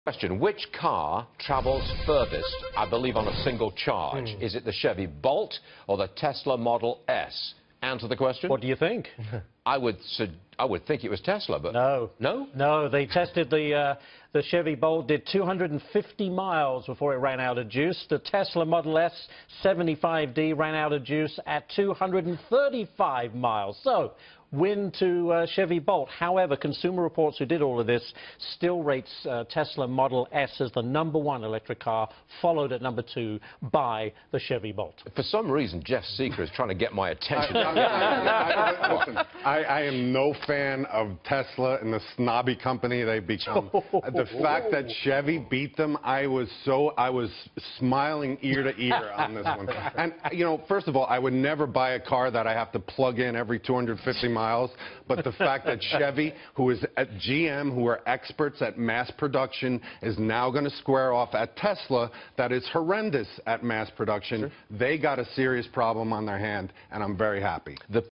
Chevy Bolt vs Tesla Model S: Which car goes furthest on a single charge?
FBN's Ashley Webster breaks down how the Chevy Bolt compares to the Tesla Model S.